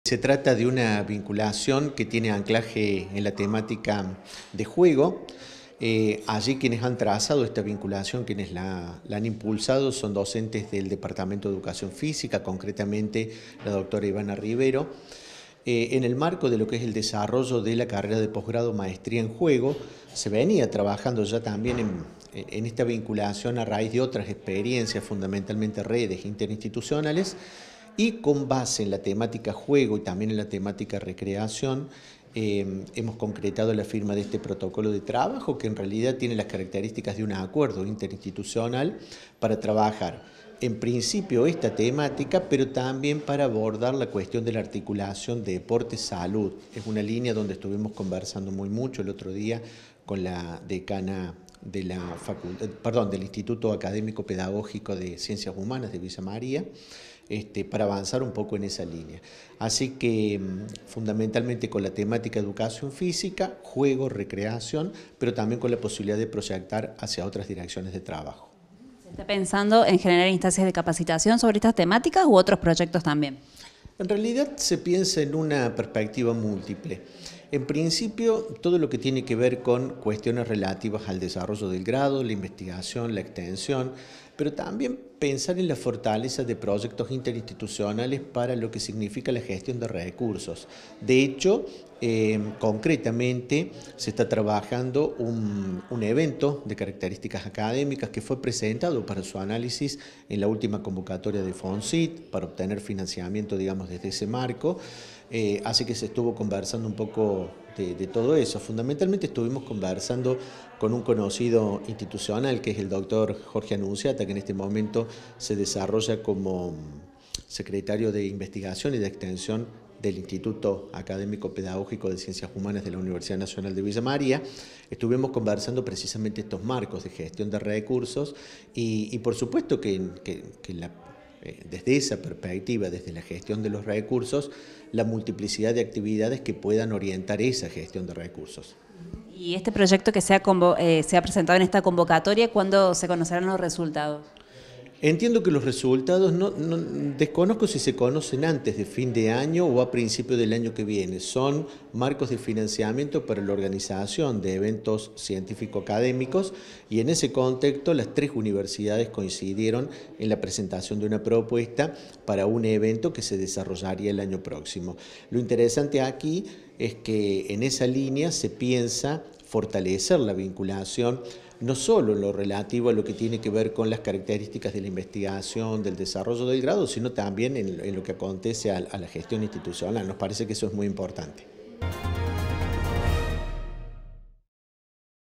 ED.FÍSICA: HUMANAS FIRMÓ UN PROTOCOLO CON LA UNIVERSIDAD DE VILLA MARÍA Y LA UNIVERSIDAD PROVINCIAL
FABIO DANDREA – DECANO FCH
UniRio TV
Universidad Nacional de Río Cuarto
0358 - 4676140